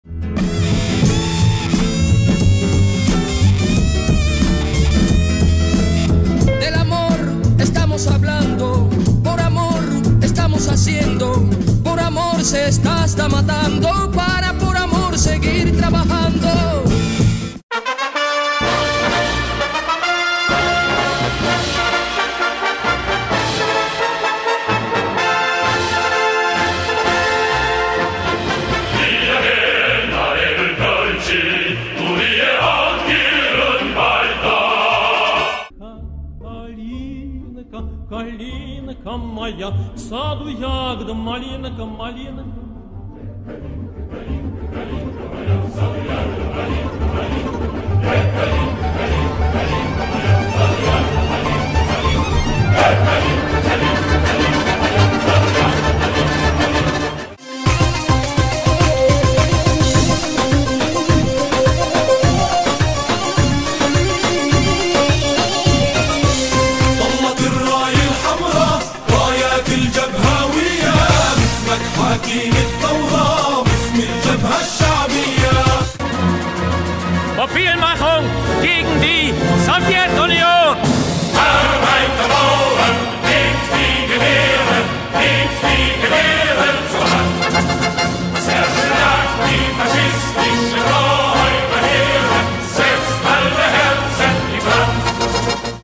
Socialist Cultures vs Capitalist Cultures
What is capitalist culture but consumerism, worshipping billionaires they’ll never be, watching the stock market crash, and petty national polarization?

Footage off YouTube